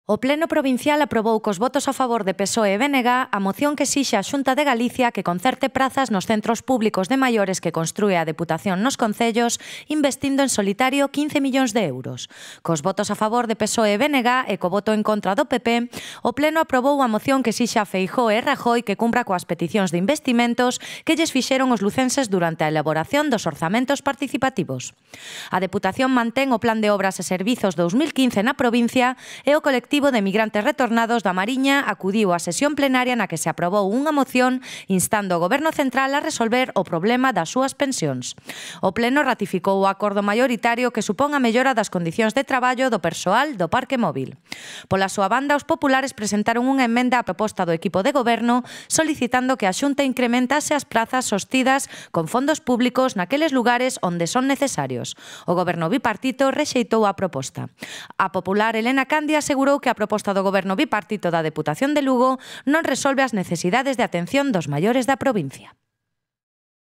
Mocion da Deputación sobre prazas concertadas de maiores.
O Pleno da Deputación aproba unha moción que esixe á Xunta que concerte prazas nos centros de maiores do ente provincial